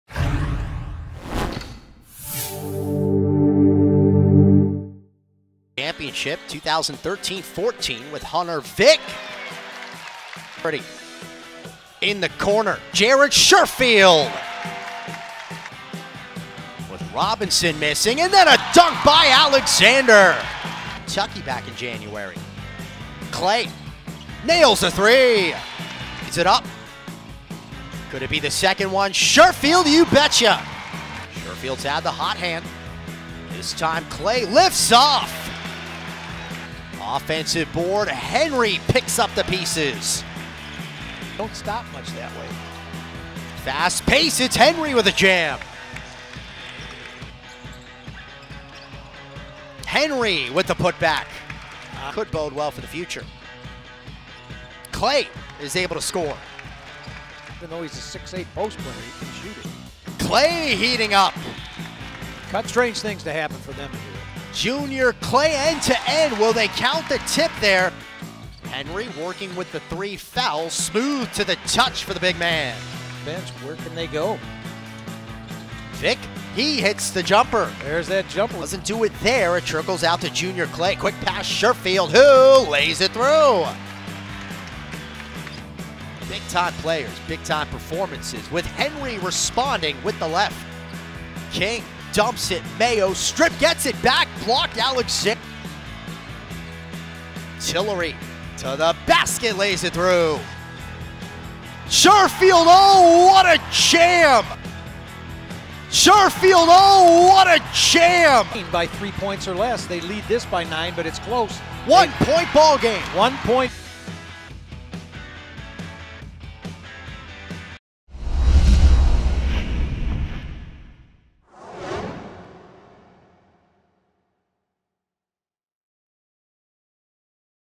Highlights: Men's Basketball vs Eastern Kentucky 2/21/19
Final:
Eastern Kentucky-67
Tennessee Tech-66